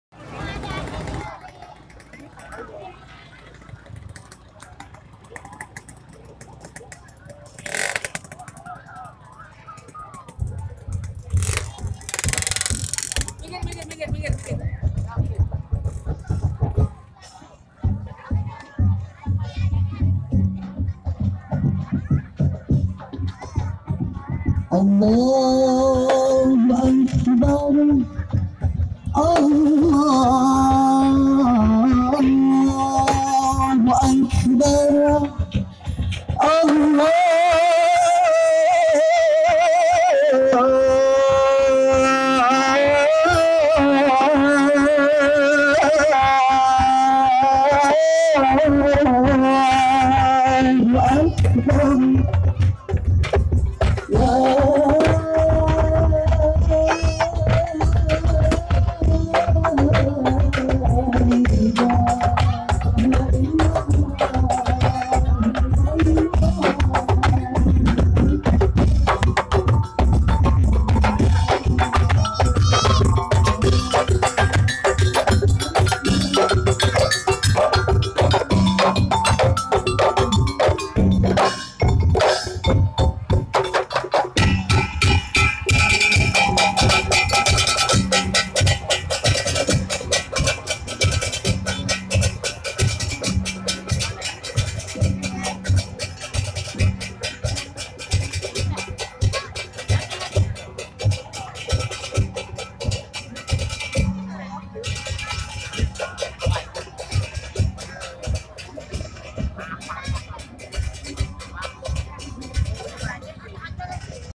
TAKBIRAN IDUL FITRI 2018M 1439H
Juara Arakan Sahur & Takbiran 2018M 1439H
1.RMA
2.gandrung
3.darussalam
4.munawarroh

1.Nurul Iman
2.tungkal ilir
3.ridwanniyah

 Juara Favorit RMA